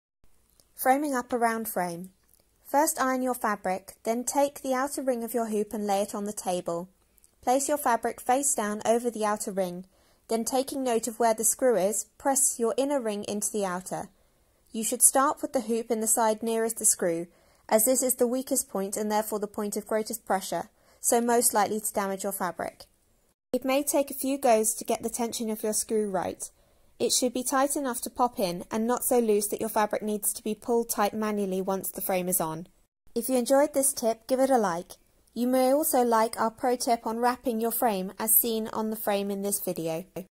Pro Tips: Framing Up a Round Frame for Embroidery/Tricks from Professional Hand Embroiderers
Framing up a frame might seem like one of the most basic things you could possibly do in embroidery, and in truth, it is. But here are a few little tips that might help to bring your embroidery up a knotch just in the preparation of your stitching:

1. Ironing your Fabric

For embroidery, you want a nice, flat, even surface on which to embroider as this will help to create consistent tension. Any creases in the fabric represent an area of fabric that has already been distorted before you begin. A crease will have lesser tension than the rest of the fabric and therefore has the potential to become a gather or pucker whilst you work therefore if you give it an iron before you begin you should be able to illiminate this risk. (Always test your fabric somewhere inconspicuous first before ironing all over and follow the ironing instructions for that fabric if provided)

2. Wrapping your Frame

Within the frame, whilst you work, you put pressure on the fabric which can change the tension. Wrapping your frame in calico or a similar fabric provides a bit more of a grippy surface for the fabric to pull against and maintain your tension (see our previous blog on this for more details).  You may also find for some fabrics that the calico provides the extra bonus of a little padding which can decrease your chances of having frame marks on your fabric when you finish.

3. Pressing on the Frame

You’ve ironed your fabric beautifully flat, and your frame is wrapped to perfection. All that is left is to put the fabric in the hoop, how you do that can also makes a difference, particulally if you have a seat or clamp frame. Put the top side of your hoop face down on a table, lie the fabric centrally over the hoop then place the inside of the frame within in. You are looking for the part of the outer hoop that has the bolt in it to be smoothly within the hoop first. This is because it is the area of highest pressure due to the bolt and the ends of the hoop being there so if this goes in first, the pressure created when we push the frame into place is displaced equally around the hole curve of the hoop rather than condensed onto these corners.

4. Respect your Frame

This last one is more of a bug bear for us, but getting into this good habit will increase the lifespan of your frame. Anytime you want to look at the underside of your embroidery, loosen the screw that attached the frame to the stick so that it can move easily. When this screw is tight, is it what is holding your frame at your desired angle to work on, so when you force it up and down, inevitabley over time the screw will loosen and no longer be able to support the weight of the frame at your prefered angle (plus it makes a horrible screech). Loosening it will give it a much better chance of serving you long and well.

Hope you found this helpful!